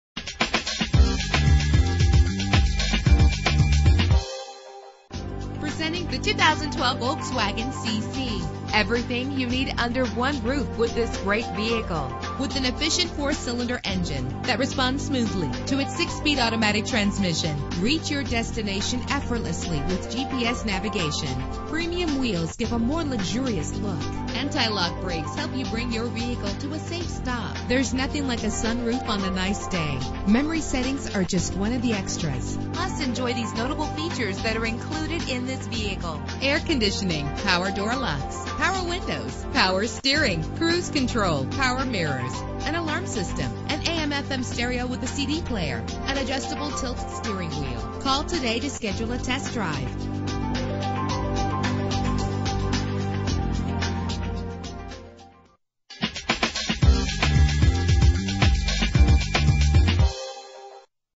2012 Volkswagen CC - Feasterville PA
Year: 2012
Make: Volkswagen
Model: CC
Trim: Lux Plus
Engine: 2.0 liter inline 4 cylinder 16 valve
Transmission: 6-Speed Automatic with Auto-Shift
Color: Reflex Silver Metallic
Mileage: 10
Address:
200 West Street Road